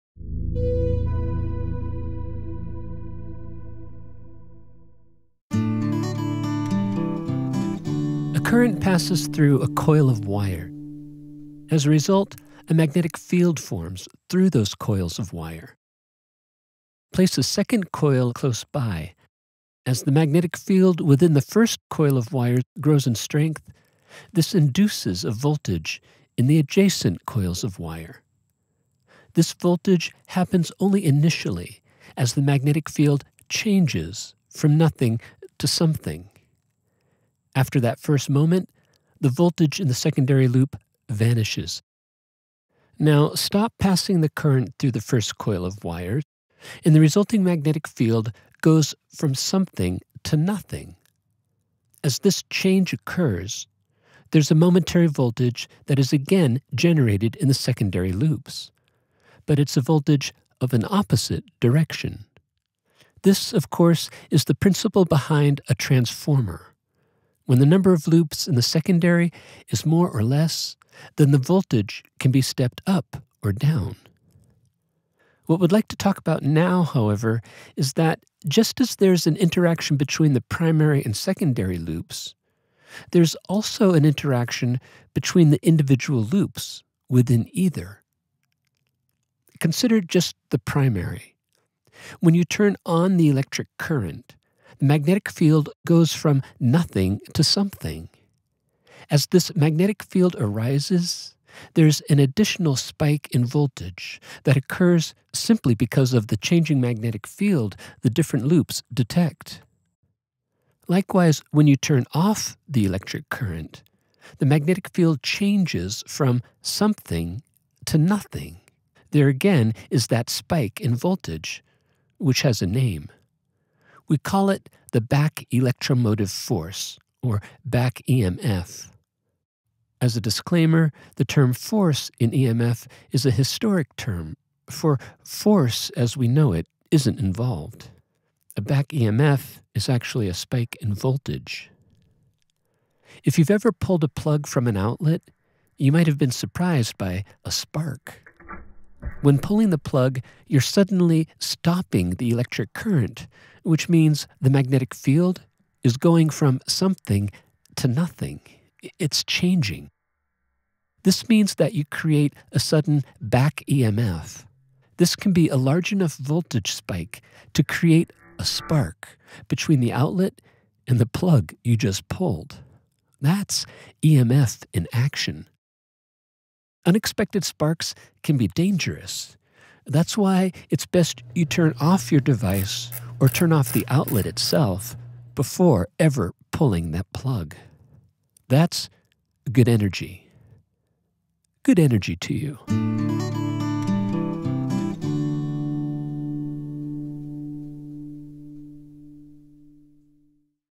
Self Induction | Primary and Secondary Voltage as Magnetic Field Form | Arbor Scientific
After a review of how a primary loop of wire can induce a voltage spike in a secondary loop of wire, this video goes on to explore self induction, which is when the interaction between the loops within a coil cause a voltage spike.